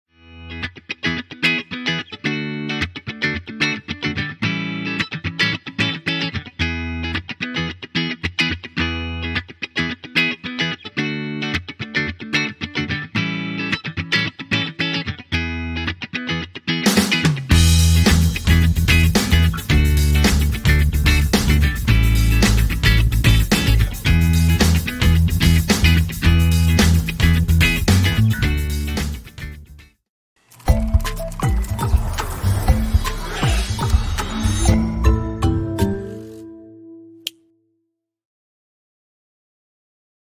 Essay Analysis - Quick Tip
A short instruction on essay analysis created by Dean Elizabeth Xyr.